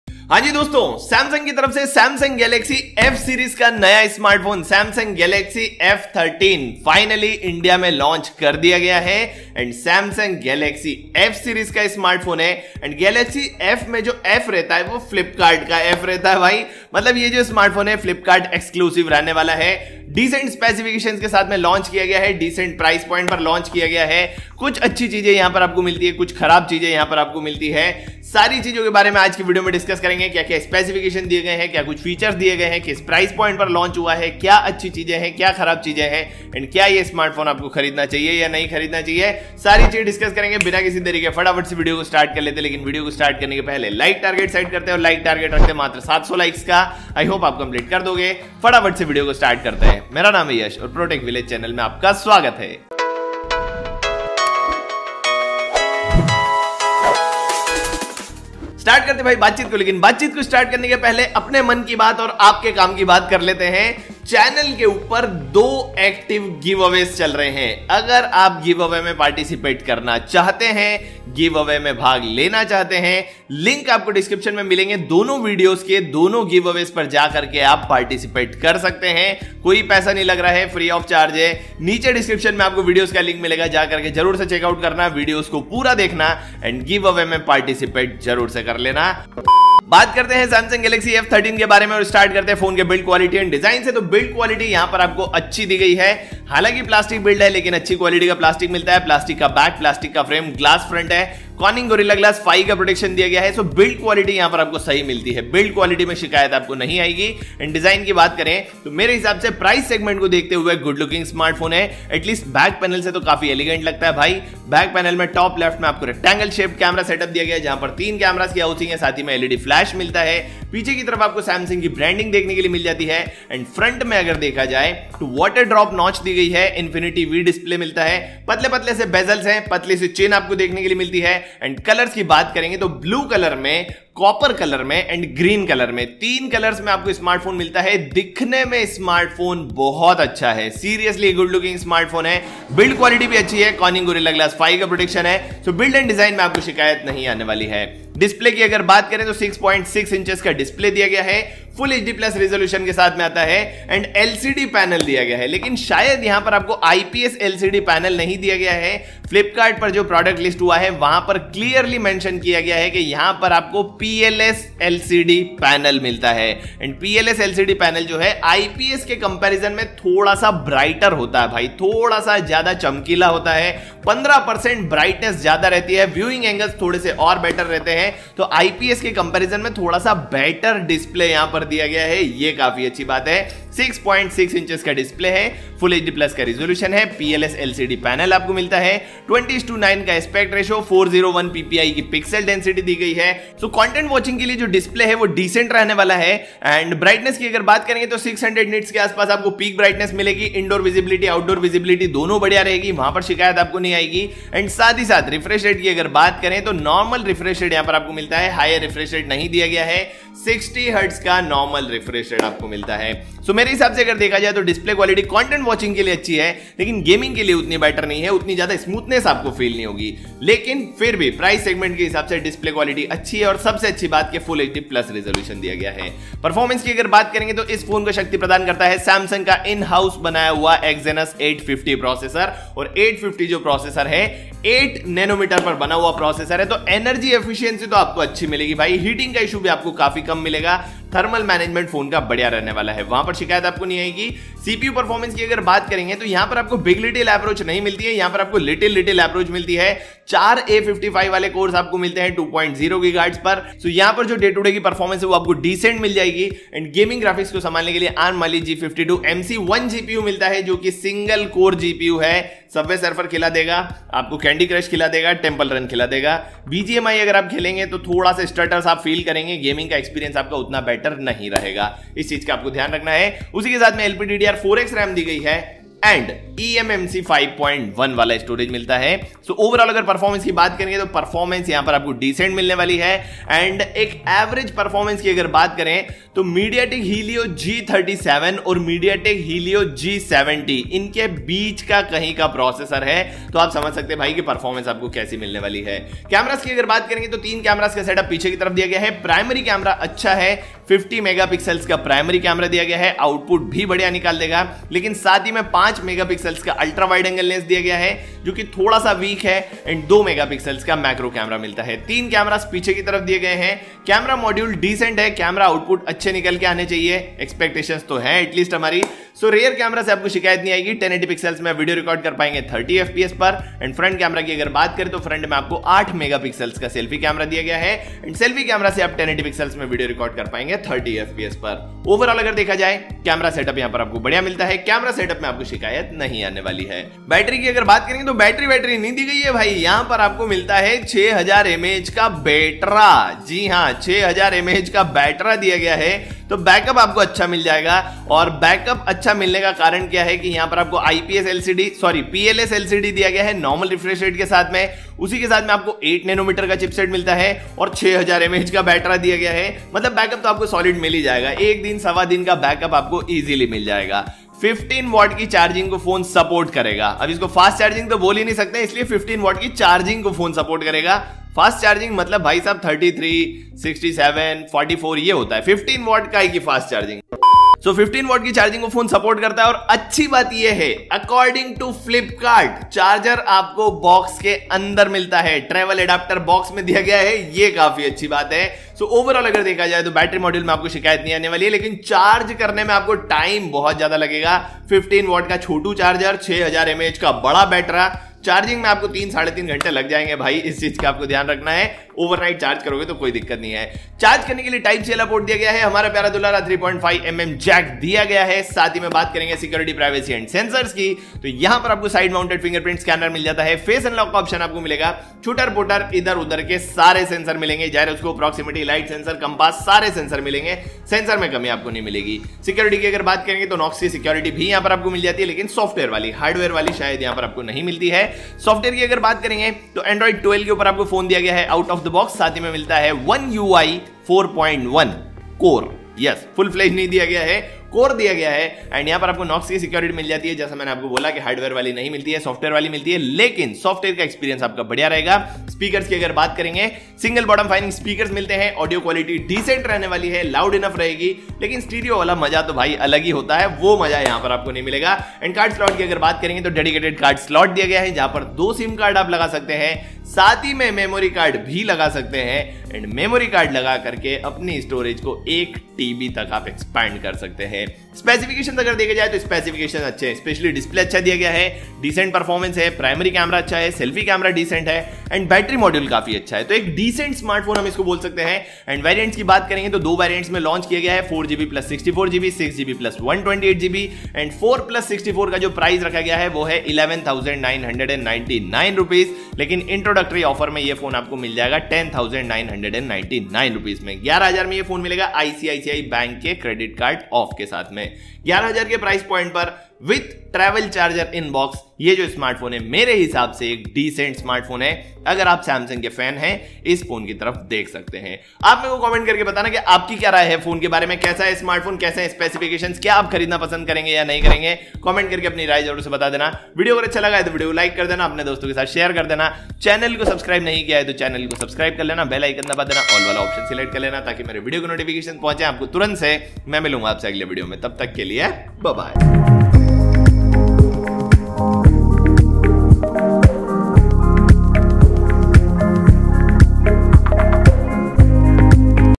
Should You Buy Samsung Galaxy F13 4G 😣😣 - Rs.10999..? [HINDI]
Hello Guys,

About this video:

Hello Guys,

Is video me humne baat ki hai Samsung Galaxy F13 4G k specifications and features ki. I hope apko ye video pasand aayega.

Thanks